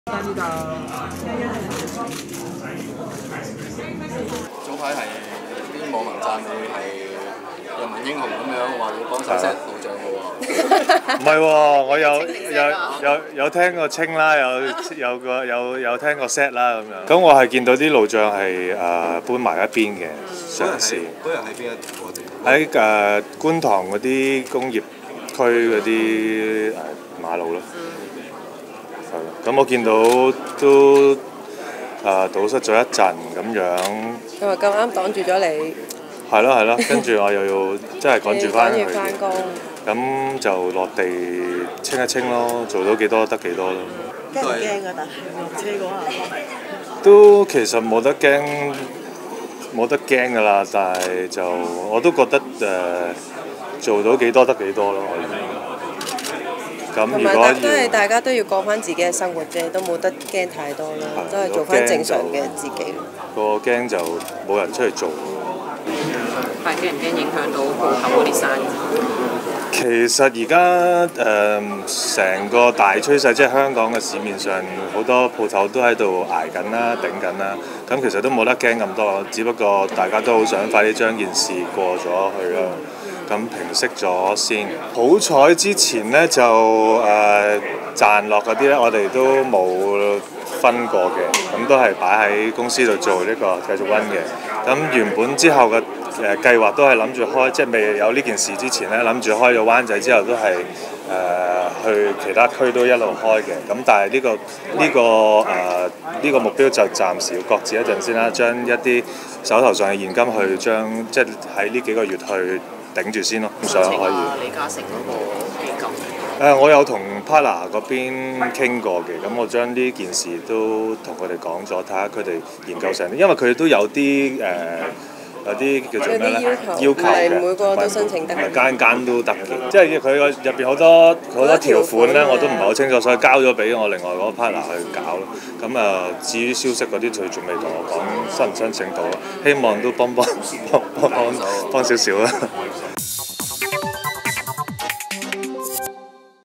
燉湯生意受影響 楊明擱置開分店︰希望事件盡快平息
楊明早前被發現在觀塘清理路障，他昨晚出席晚宴時憶述當日返公司，受路障影響塞車45分鐘，由於趕時間就下車幫手。

他說︰「當時沒有想太多安全問題，做得幾多得幾多。如果人人都怕，就沒有人出來做了。大家都要生活，下次見到會繼續清理。（傳你幫手設路障？）網上有很多『看圖作文』，沒想到不幸之中也有點笑料。」

提到其燉湯店生意，楊明坦言已擱置開分店計劃，他說︰「全港舖頭也在捱，希望事件盡快平息。幸好之前的盈利沒有攤分，可以暫時用來周轉。我和拍檔正研究申請李嘉誠基金會資助中小企的基金，希望可以幫補一下。」